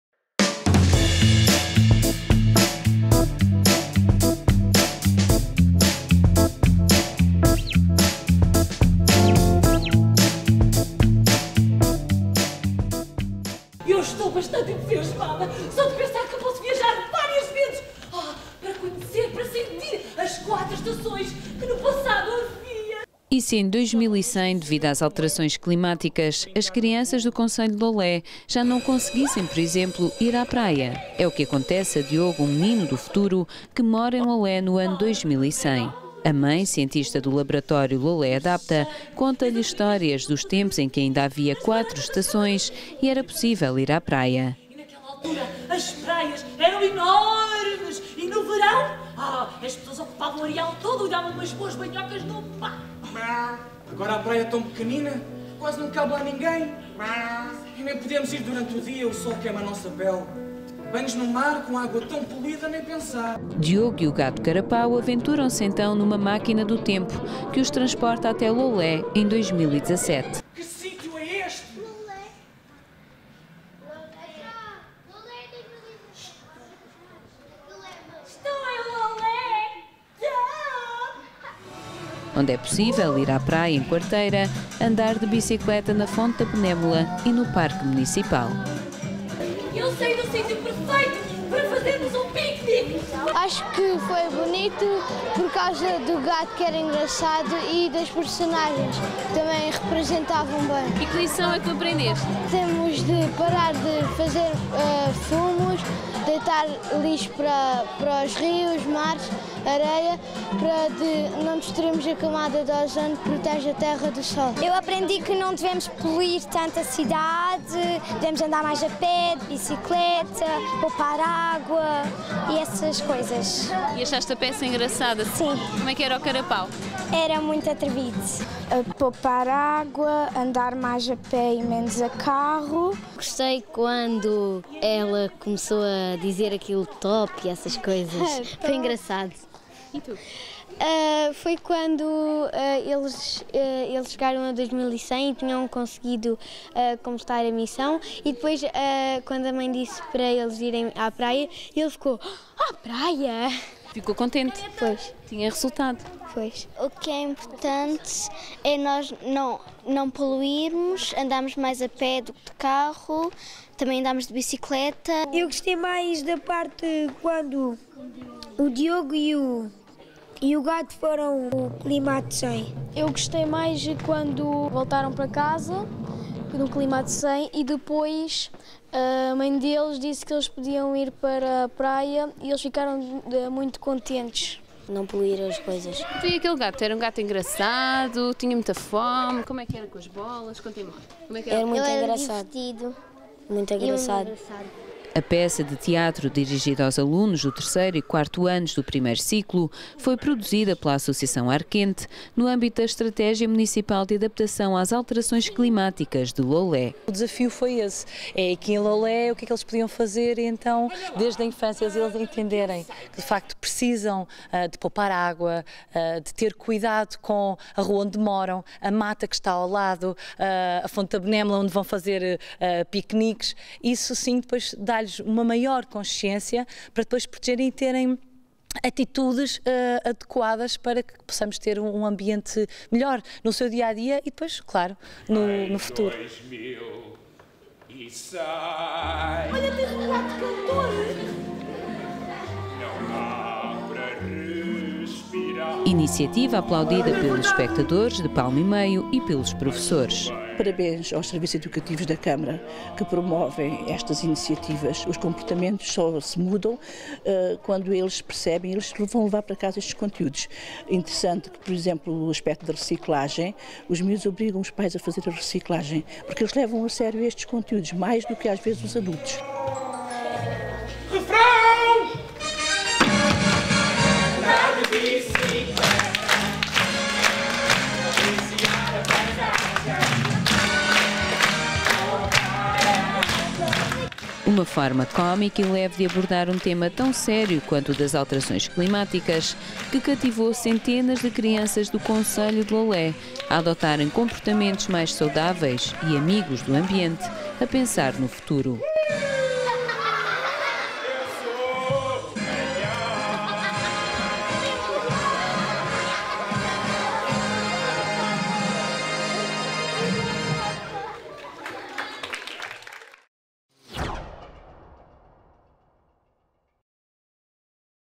Peça Infantil no âmbito da Estratégia Municipal sobre as Alterações Climáticas no Concelho de Loulé
Produção: Darquefalar
Coordenação: Loulé Film Office 2017